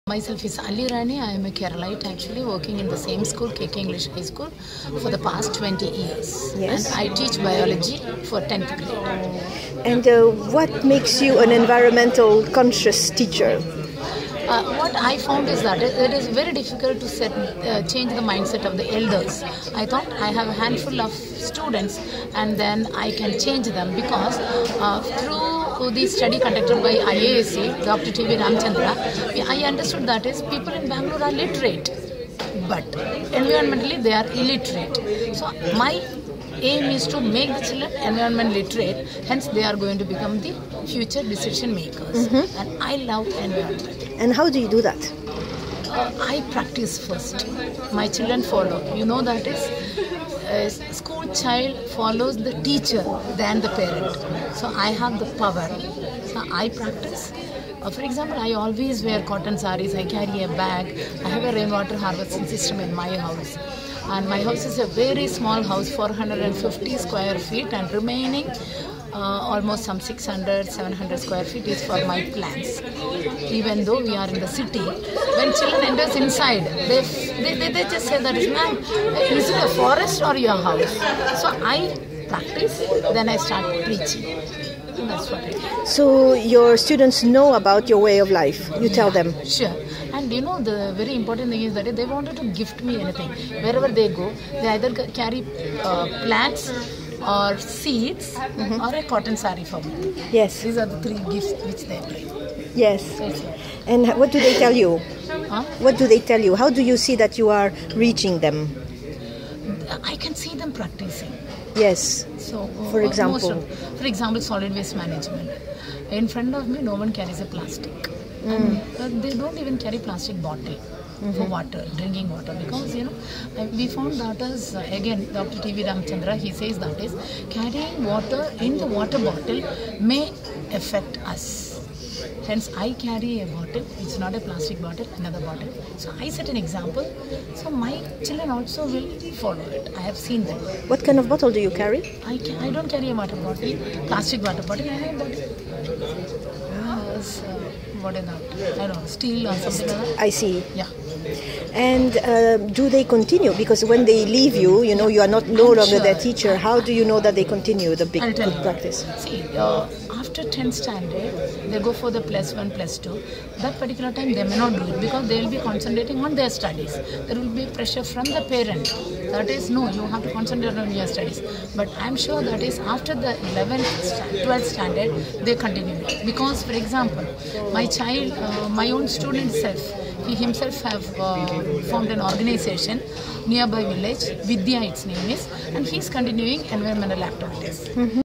Conversation with Ms. Alli Rani, biology teacher at K K English High School in Bangalore
K K English High School, Varthur, was awarded the prestigious Vimal Kshetra Award 2016 instituted under the banner of United Nations University-UNU-MERIT Netherlands and Indian-French NGO Friend in Need India. The award was presented at a conference held at New Delhi on November 19 and 20. The national award was given under the "Change Makers" category. This award is given in recognition of all the initiatives taken up by the school in environment conservation as well as in creating environment literacy amongst the students, parents and the community.
Principal M A Khan and science teacher Alli Rani V P received the award on behalf of the school.

Source: National Award For Green Initiatives Presented, Times of India, 15 December 2016